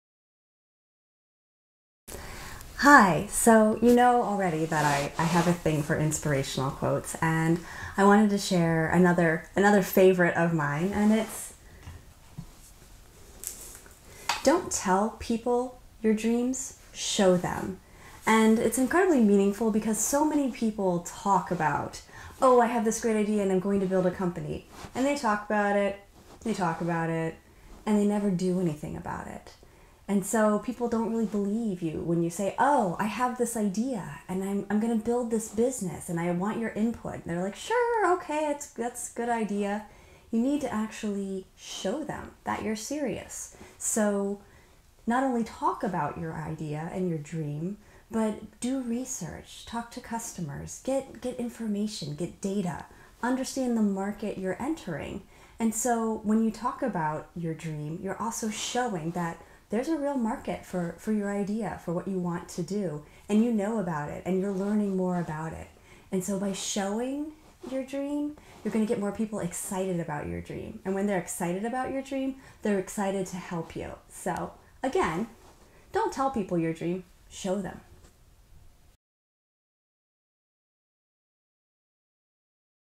Don't Tell People Your Dreams, Show Them
Entrepreneurial insights from Danielle Tate, Author of Elegant Entrepreneur